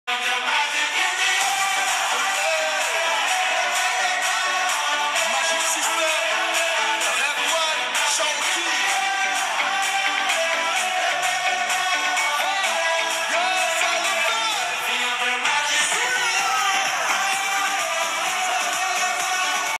duo/trio baton juniors - Marijana Tucak and Branka Tucak - Split, Croatia
Marijana Tucak, Branka Tucak
Split, Croatia
duo/trio baton juniors